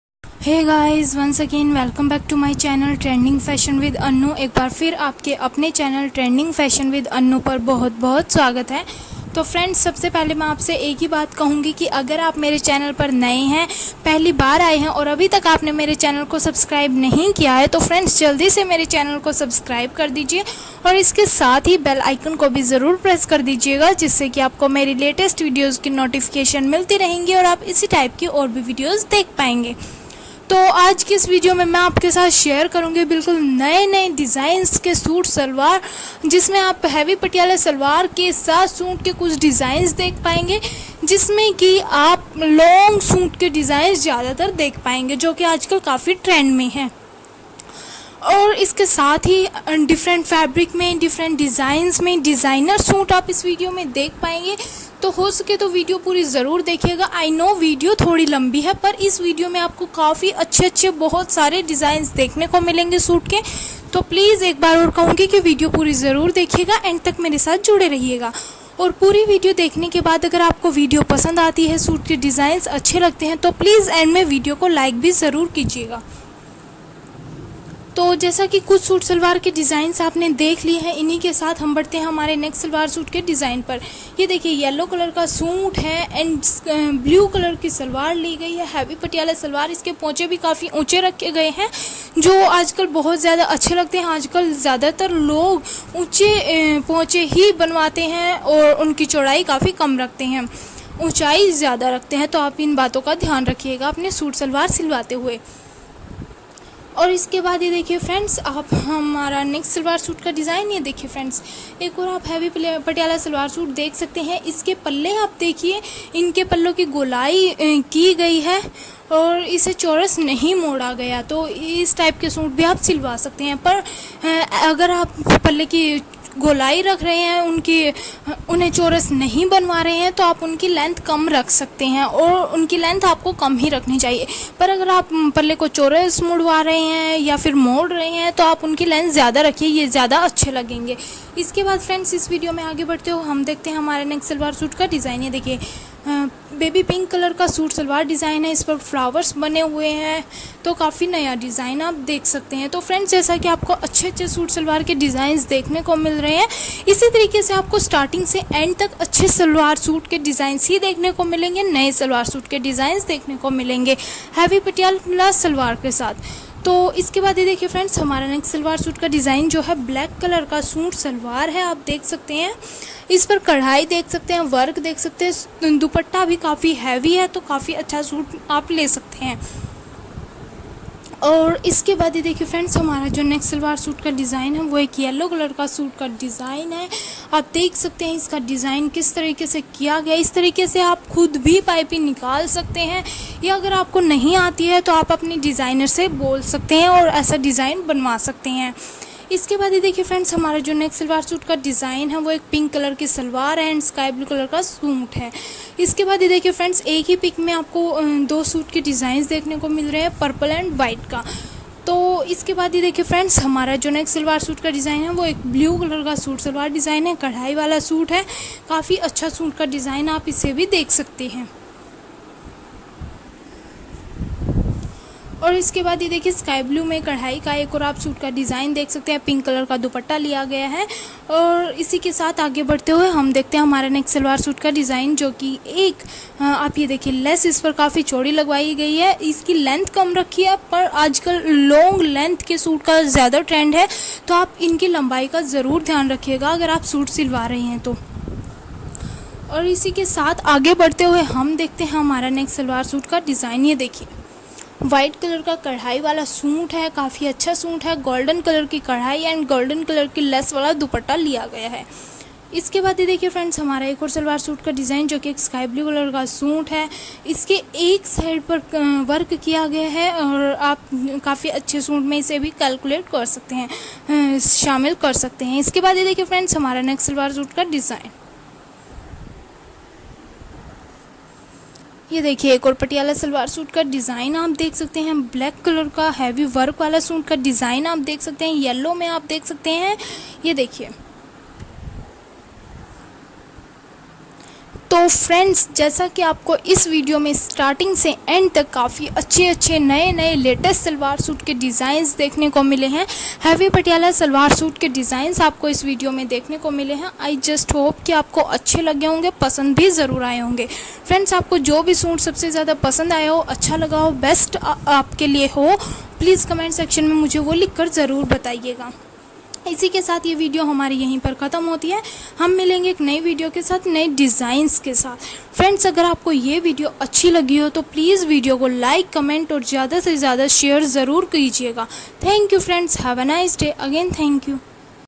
Latest top new heavy patiyala salwar suit designs 2020 || बिल्कुल नए सलवार कमीज के डिज़ाइन 2020-2021
Hii friends.

I am Annu Deswal.

Welcome back on my youtube channel "Trending Fashion with Annu."

Today in this video, I will show you the beautiful designs of punjabi heavy patiyala salwar suit designs 2020. I hope that you will like it.

Thank you for supporting me also.